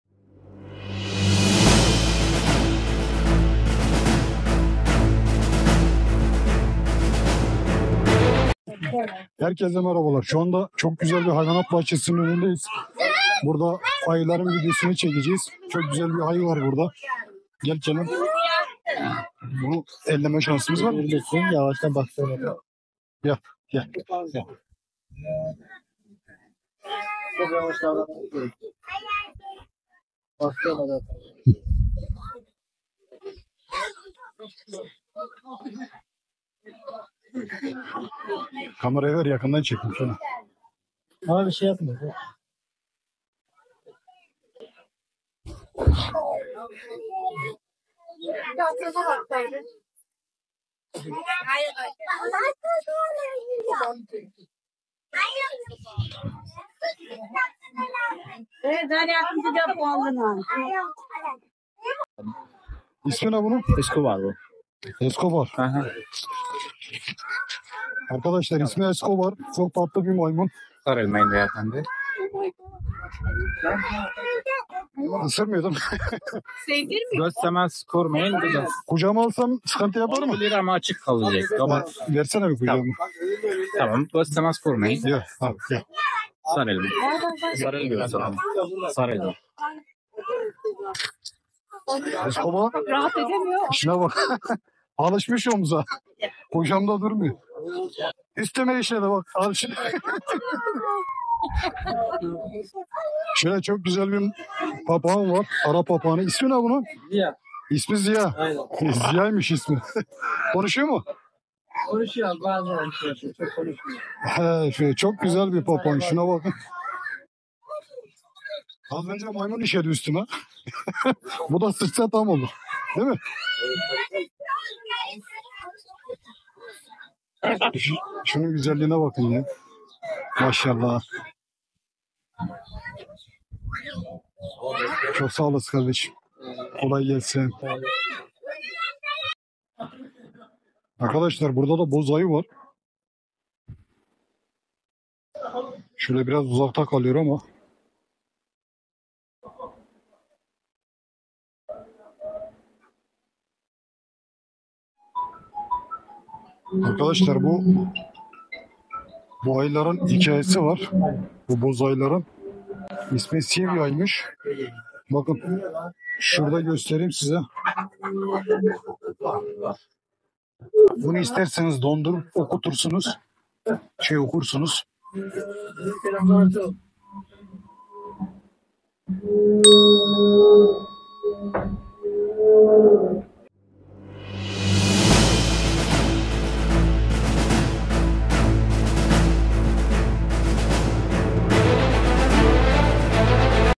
HAYVANAT BAHÇESİ GEZİSİ! ÜSTÜME MAYMUN İŞEDİ 😂
Çekmeköy hayvanat bahçesi gezisi videoların devamı gelecek.